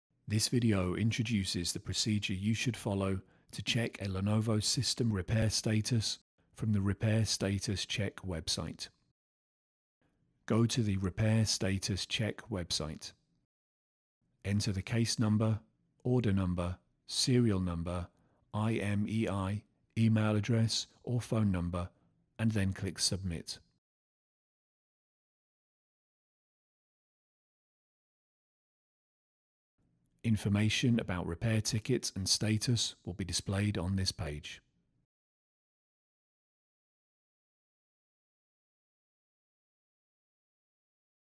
How to check the Lenovo system repair status from the Repair Status Check Web site
This video introduces the procedure you should follow to check the Lenovo system repair status from the Repair Status Check Web site.